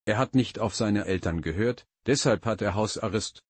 How to pronounce Er hat nicht auf seine Eltern gehört,deshalb hat er Hausarrest German Pronunciation
"Hello all!
Our German word of the day is 'Er hat nicht auf seine Eltern gehört, deshalb hat er Hausarrest.'
It means 'He didn’t listen to his parents and therefore he is on house arrest.'